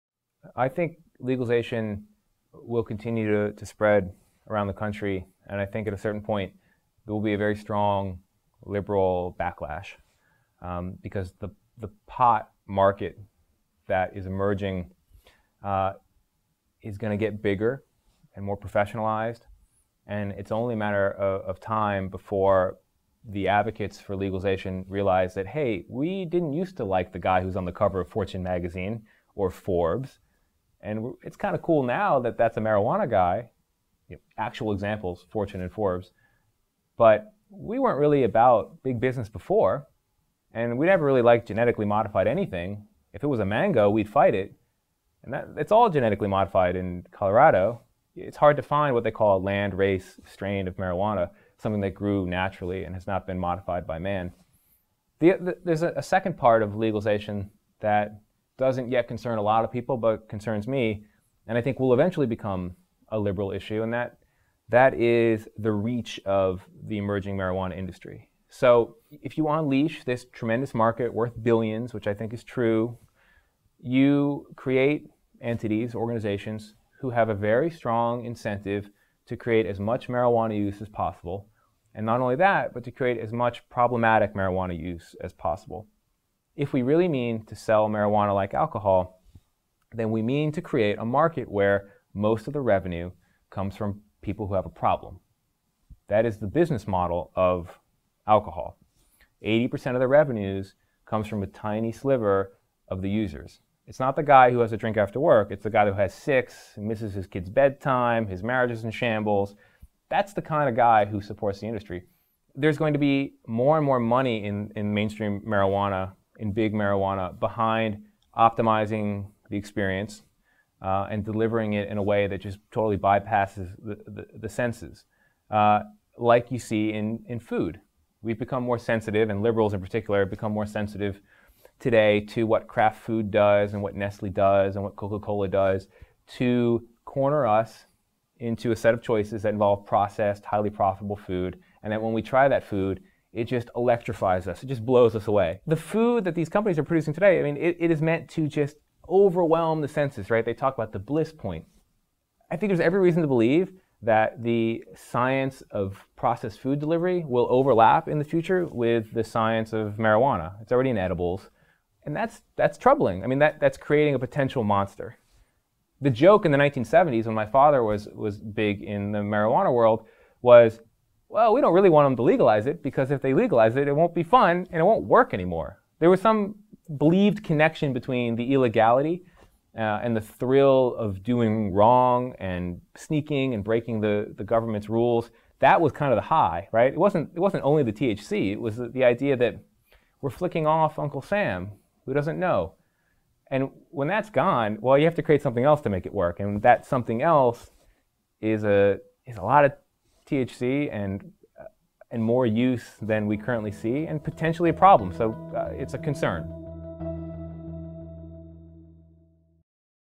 Tony Dokoupil on the Coming Marijuana Backlash | Big Think
Tony Dokoupil on the Coming Marijuana Backlash
America has been undergoing a marijuana revolution of sorts. Marijuana is legal for medical use in 20 states and the District of Columbia, and now it’s sold in Colorado and Washington, with around a dozen more states likely to follow. A recent study by the University of Texas at Dallas found that legalized medical marijuana actually lowered crime rates.

This is all good news for advocates for legalization, but author Tony Dokoupil warns of a coming backlash. What will be the impact once marijuana is mainstream and industrialized? Dokoupil, the author of The Last Pirate: A Father, His Son, and the Golden Age of Marijuana, raises important arguments that should be of concern to people on both sides of the issue.
Tony Dokoupil:

Tony Dokoupil is a senior writer for NBC News and the author of The Last Pirate (Doubleday, April 2014), a book about a father, his son, and the golden age of marijuana.

Over the years, his reporting has made The Daily Show (the moment of zen) and The Tonight Show (the opening monologue), and generated guest spots on CNN, NPR, MSNBC and The Today Show. His writing—which has recreated drug smuggling, hate crimes, suicides, and free sperm donations (not a euphemism)—has garnered attention from the good people at Longform, Byliner, and Longreads.

HBO awarded a development grant to turn his story “The Devil in Deryl Dedmon” into a documentary film, and NPR's Fresh Air made his piece "The New Pot Barons" into an hour long program. His earlier cover story “The Coffee Shop Baby” became a 30-minute segment on ABC’s 20/20, and his profile of the antiquities-hunter Forrest Fenn helped push the man’s hidden treasure into the pantheon of findable millions. His 2009 story “My Father the Dope Dealer” was bought by Doubleday, and the accompanying video was a "Must See" pick of the New York Times Lens blog.

His work has taken him to more than a dozen states and into interviews with a $10,000-a-day corporate psychic, the last surviving veteran of World War I, and the only black father in Baltimore to adopt a white girl. It has included training as a private astronaut, diving to the bottom of the Pacific, and living inside the last wholly company town in America. His past characters include the reclusive author of the Anarchist Cookbook, defrocked mega-pastor Ted Haggard, and (via an exclusive cache of letters) the would-be savior of humanity, L. Ron Hubbard.

Previously, as a senior writer at Newsweek & The Daily Beast, he wrote numerous cover stories, including "The Suicide Epidemic," "iCrazy" and "Dustoff 73," which helped anchor the first annual Hero Summit in Washington, DC. His story “The Last Dive" and the original video became Newsweek's first animated cover.

Dokoupil was one of two staff writers in Newsweek's Periscope section, which was nominated for a 2008 National Magazine Award for best stand-alone department. Later he edited a weekly page of idea-driven news and the My Turn column. Dokoupil joined Newsweek part-time in the summer of 2006, working as a researcher in the international edition before moving to the domestic arts section as a full-time reporter. Prior to Newsweek, he co-wrote the “Research Report” column for the Columbia Journalism Review. His freelance work has appeared in Radar, W, and GQ.

He received his B.B.A. summa cum laude from George Washington University, where—and this just sounds immodest no matter what—he graduated first in his class. He was also captain of the baseball team, which it must be said is terrible now but wasn’t when he was there, he swears. He holds a masters degree in American Studies from Columbia University, and earned a fellowship towards a PhD in media studies before dropping out after two years to pursue journalism
TRANSCRIPT:

Tony Dokoupil:  I think legalization will continue to spread around the country and I think at a certain point there’ll be a very strong liberal backlash because the pot market that is emerging is gonna get bigger and more professionalized.  And it’s only a matter of time before the advocates for legalization realize that hey, we didn’t used to like the guy who’s on the cover of Fortune magazine or Forbes.  And it’s kind of cool now that that’s a marijuana guy.  Actual examples Fortune and Forbes.  But we weren’t really about big business before and we never really liked genetically modified anything.  If it was a mango we’d fight it.  And it’s all genetically modified in Colorado.